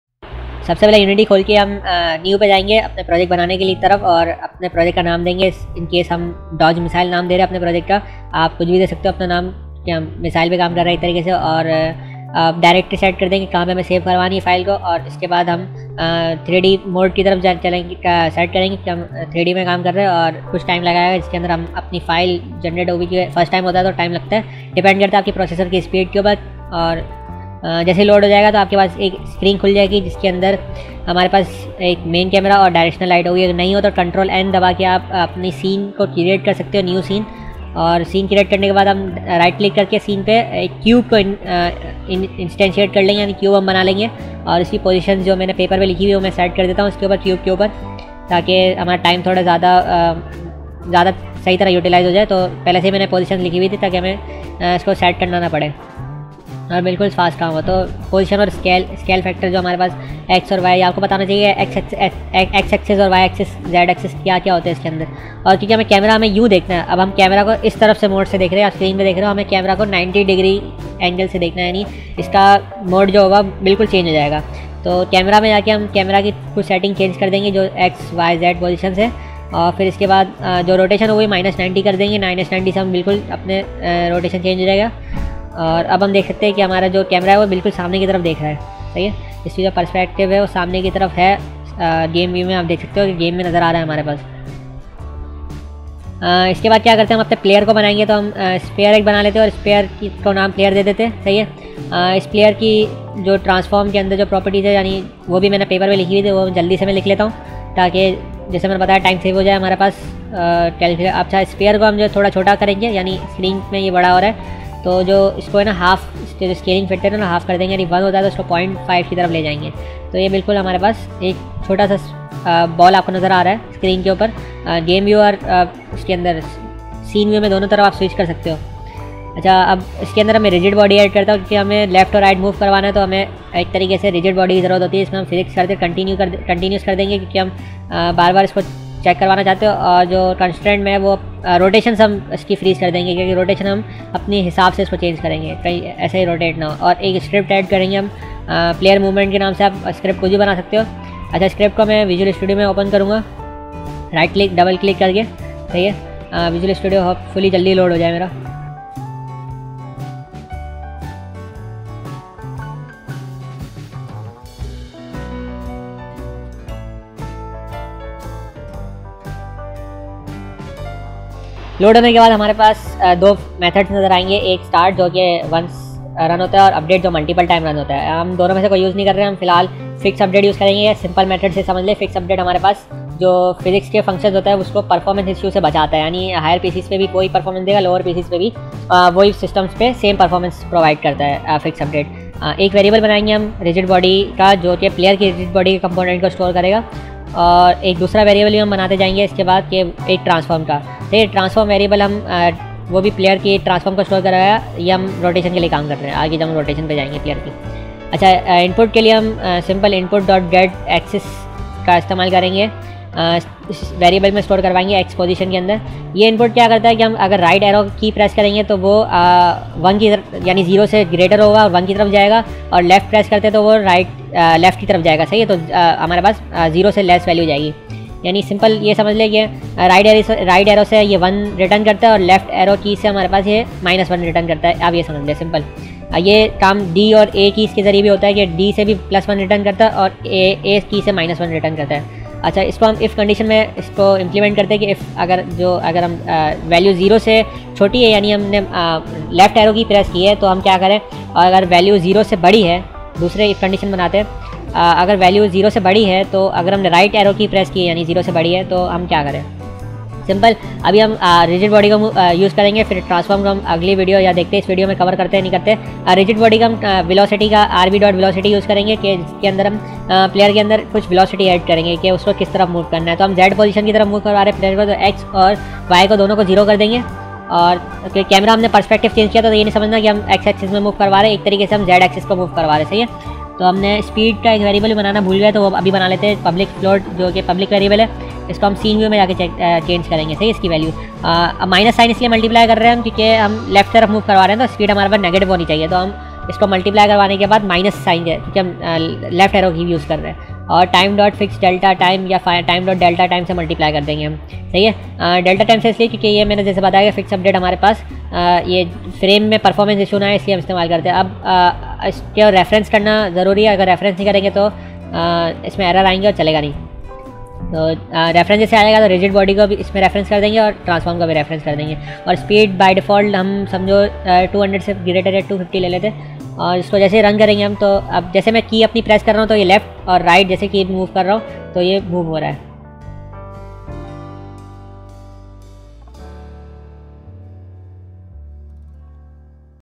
2. Unity Game Development: Dodge Missile Series
This video is all about setting up environment and player movement script. Applying some physics so our player(Ball) can move.